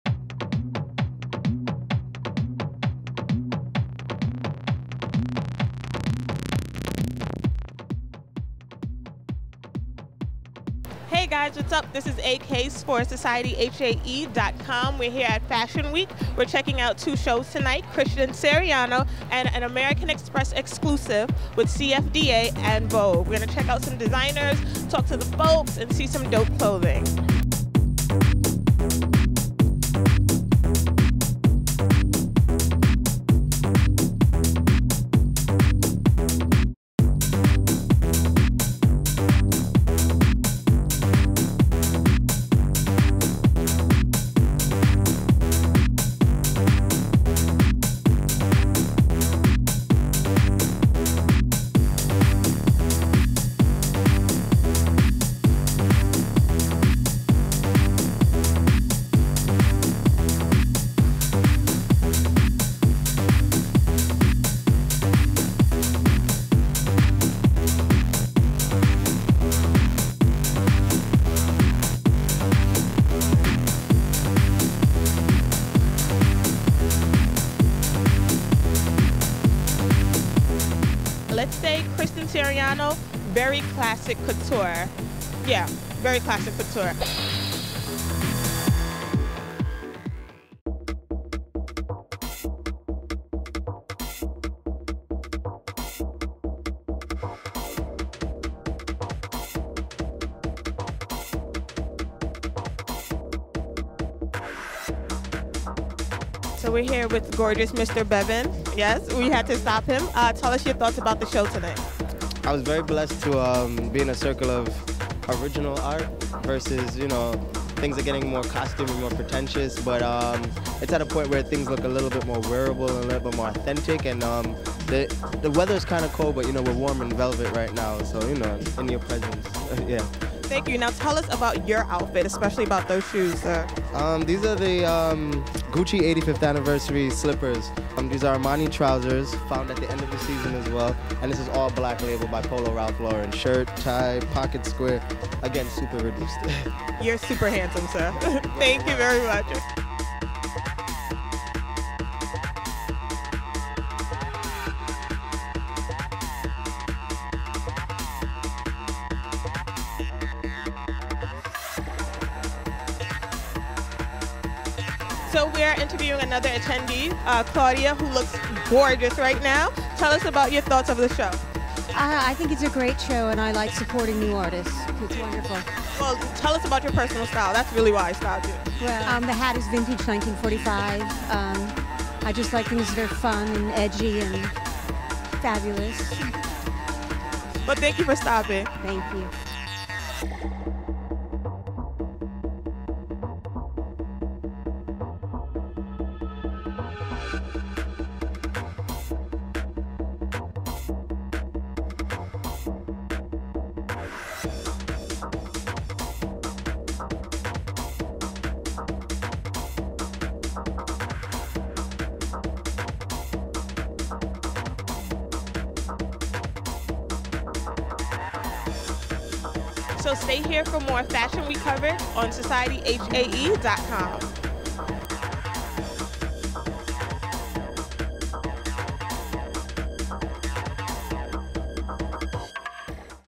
Fashion Week Kick OFF!!!
Society Hae Kicks off fashion week with Christian Siriano and an American Express exclusive with CFDA and Vogue...